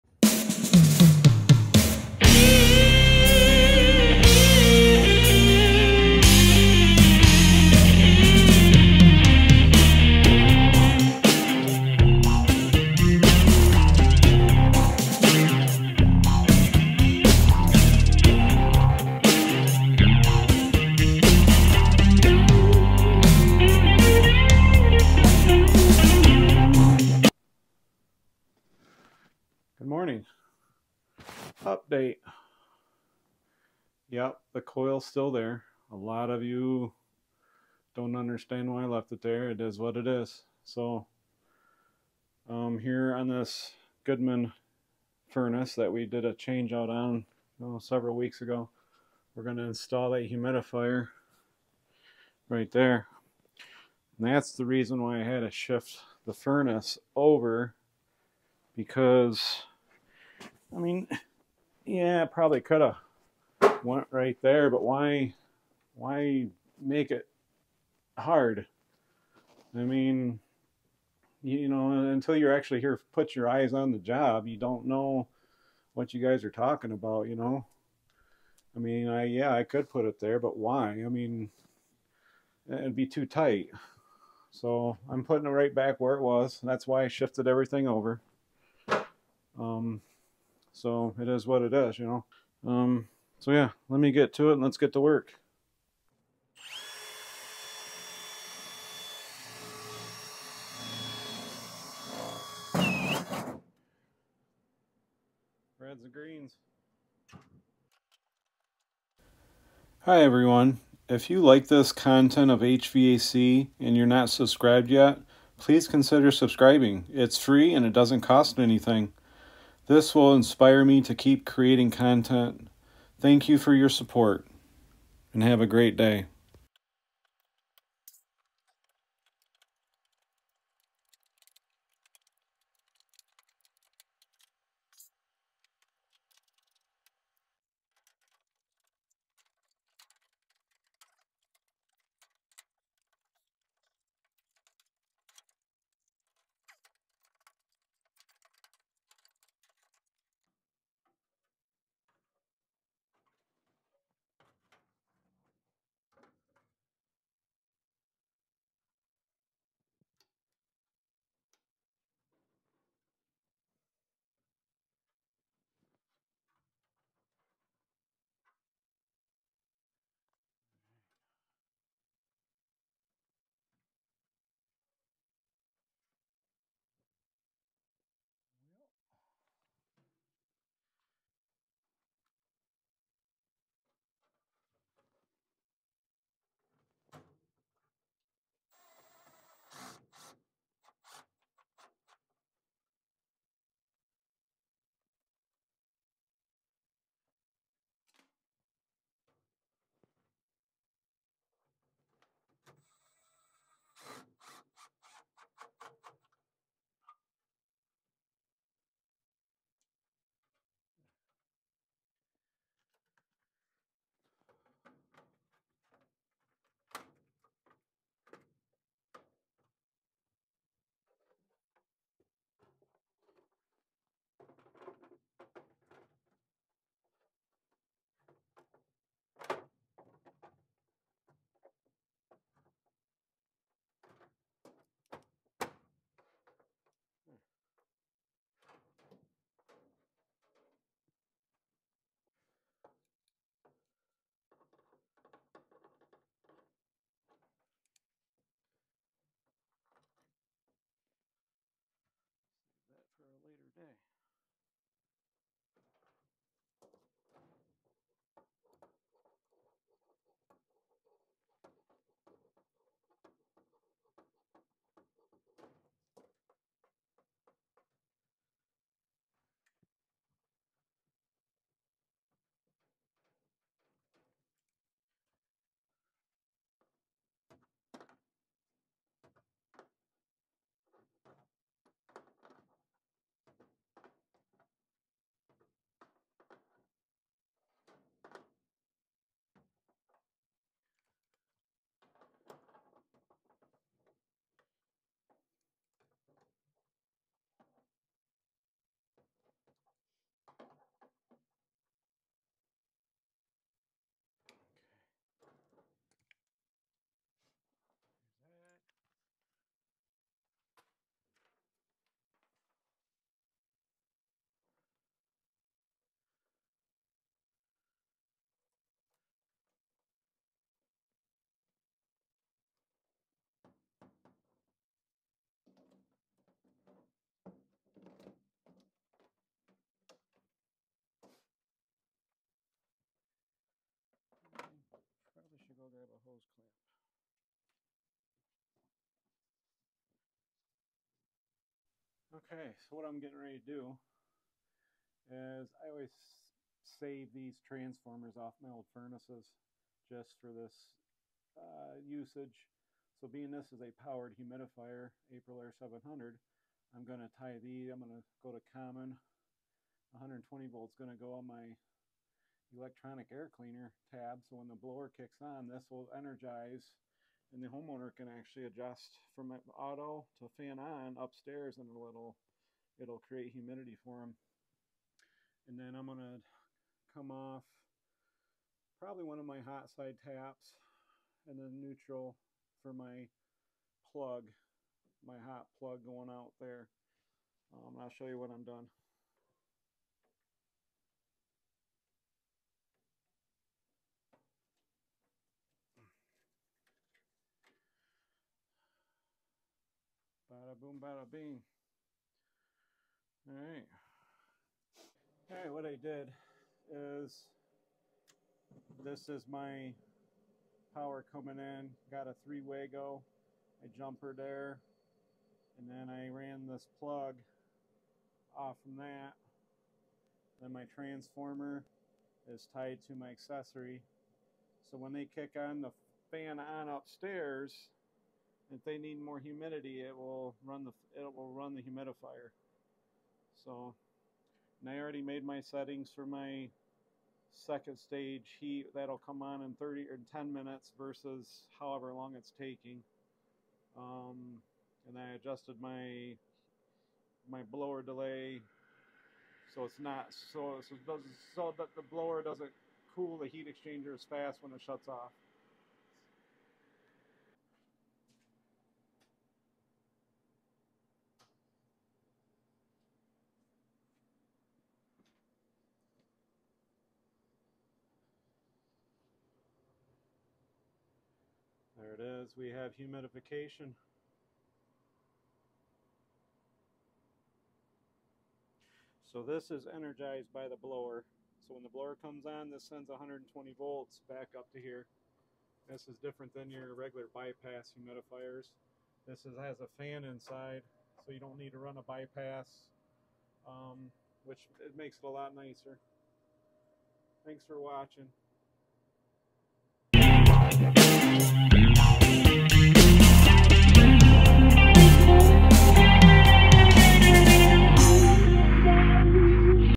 Installing a AprilAire Model 700 Humidifier
Installing an AprilAire Model 700 Fan-Powered Evaporative Humidifier
Whole-House Humidifier

A fan-powered, duct-mounted humidifier adds balanced humidity to your whole home. The fan pulls air directly from your furnace, moisturizing it before returning it to the air ducts to be dispersed throughout your home.

Please leave any thoughts, feedback, comments,  Constructive criticism, or advice in the comment section.